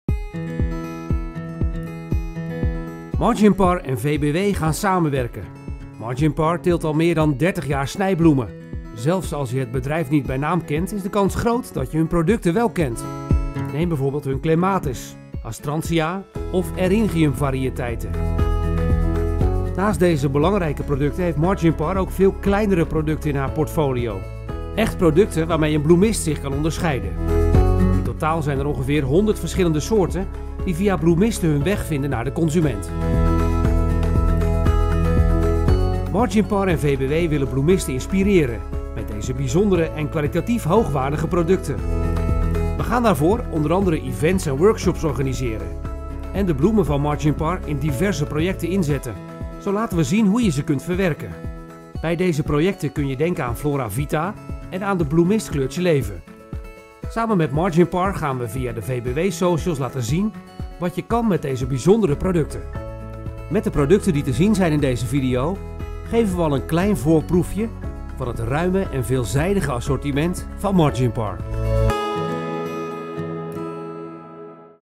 Marginpar en VBW gaan samenwerken!
Via events en diverse projecten gaan we jou het exclusieve en bijzondere assortiment van Marginpar laten ervaren.